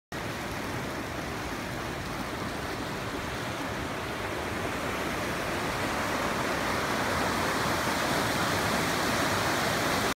Rock Creek at the Rock Creek Lake inlet
Rock Creek at the Rock Creek Lake inlet, 2017. Heavy flows this year with the high snow pack.